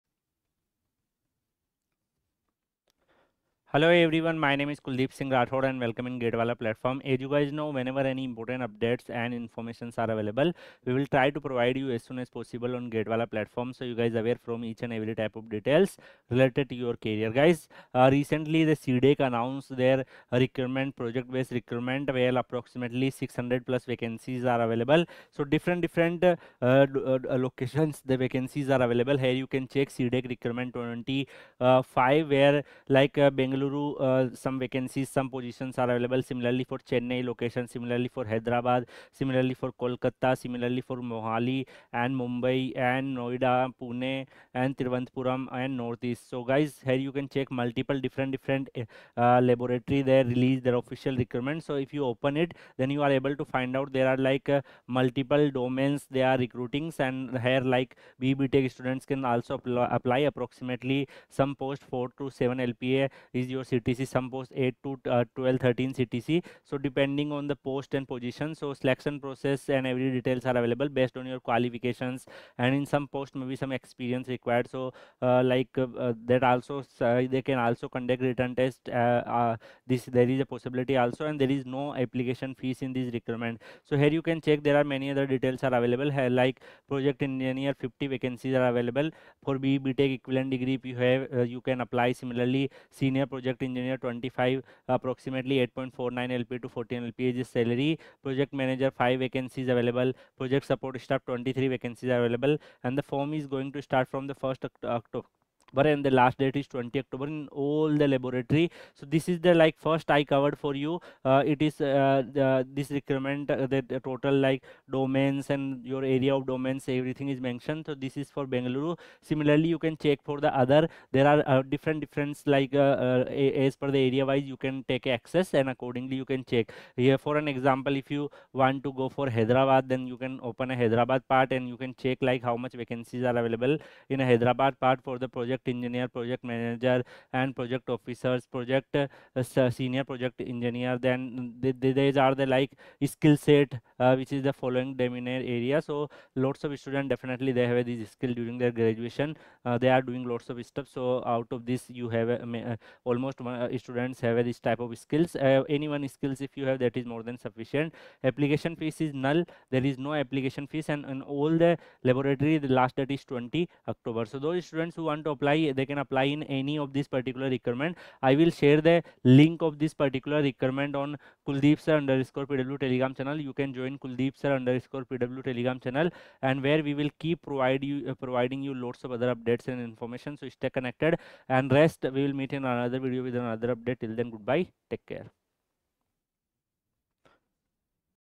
CDAC Recruitment 2025 | 600 Vacancies | Salary + Allowances | Golden Opportunity For Engineers
CDAC Recruitment 2025 is here with a massive 600 vacancies, offering an incredible career opportunity for engineers and tech professionals across India. This is your chance to secure a prestigious position with the Centre for Development of Advanced Computing (CDAC) and enjoy an attractive salary package along with various allowances and benefits. In this video, we cover the complete details of the notification, including eligibility criteria, application process, important dates, selection procedure, salary structure, and more.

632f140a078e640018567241